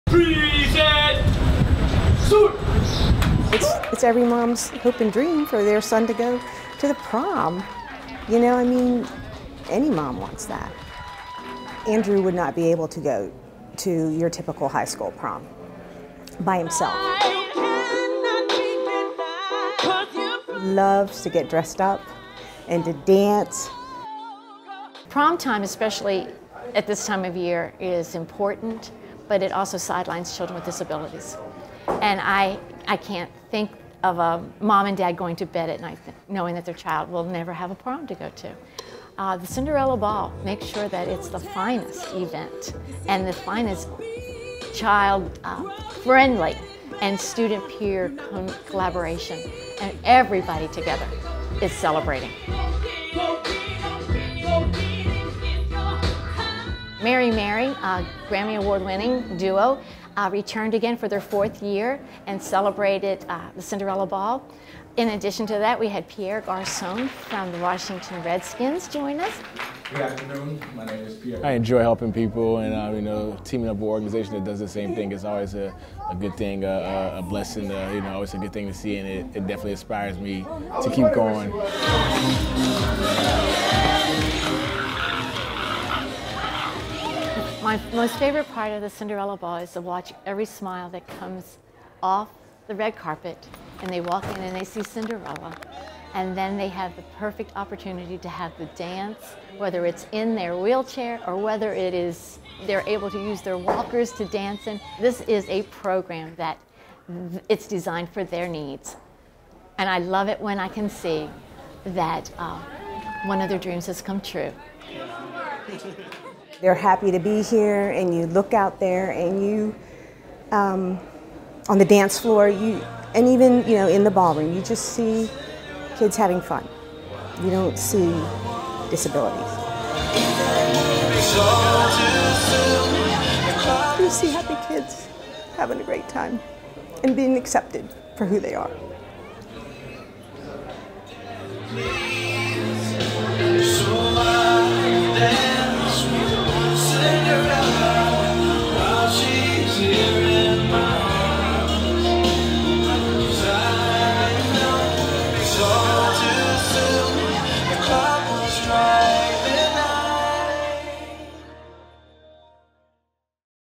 Marines Make Dreams Come True
Marines from Quantico, Va. escort students from The House, Inc. down the red carpet during A Cinderella Ball June 8, 2014. The House, Inc. Student Leadership Center programs work to ensure that all young people graduate from high school with college access. President and founder, Helen McCormick, started A Cinderella Ball 8 years ago to benefit children with life-threatening illnesses or disabilities of military families. (U.S. Marine Corps video by Cpl. Ally Beiswanger/Released)